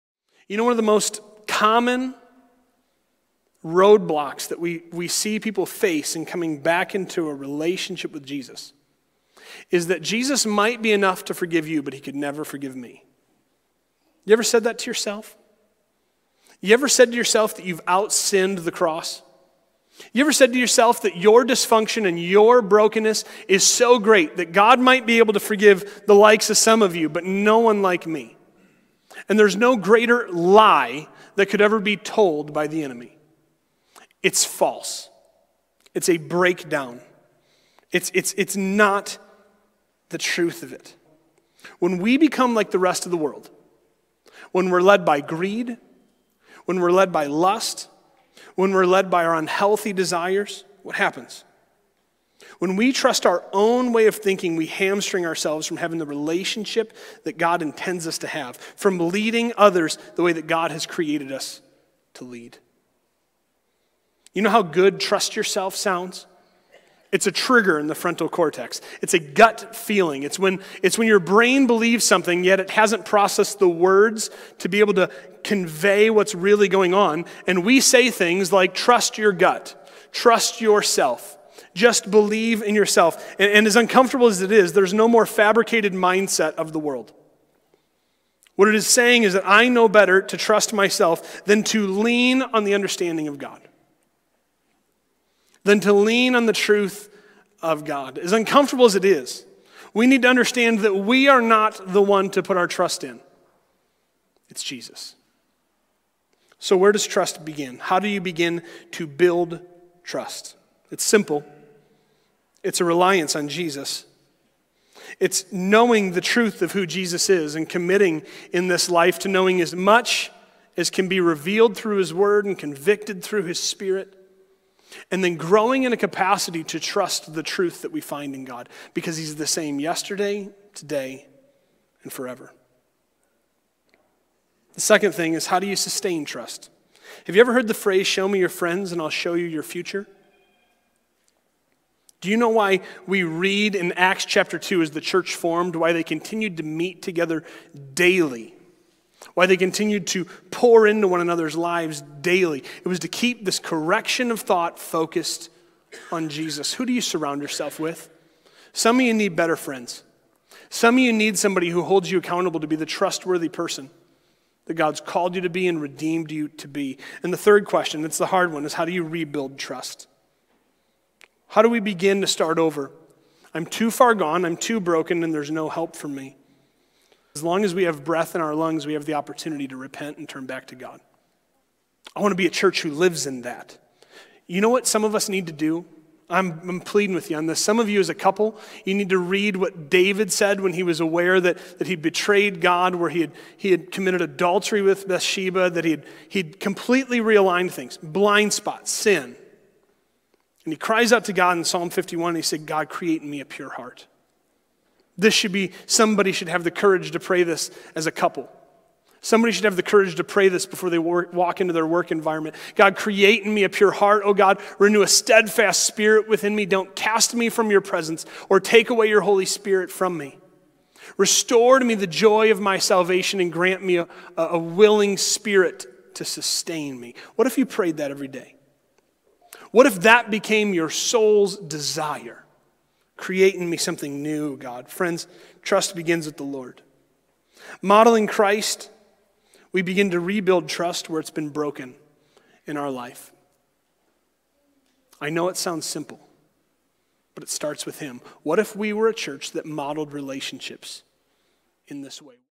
How to Build and Sustain Trust
How do you begin to build trust? It could be a new relationship or a broken relationship, either way trust is a big issue and important in building a strong foundation. This video is about the only way to build trust, which is a reliance on Jesus and knowing the truth of who He is. As long as there is breath in your lungs, there is an opportunity to come back into a relationship with Jesus.